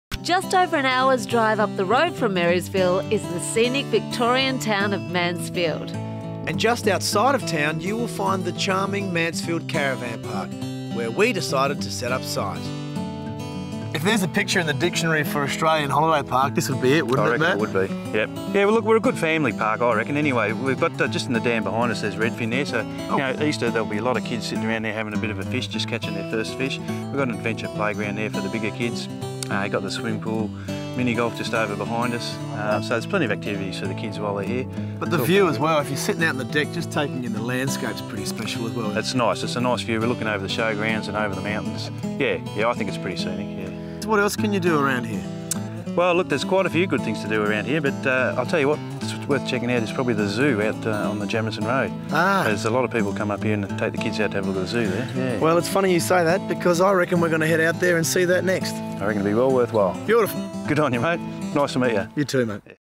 WUDU S7E05: Horsing around at Mansfield Holiday Park
Terry chats to Matt Leggett from Mansfield Holiday Park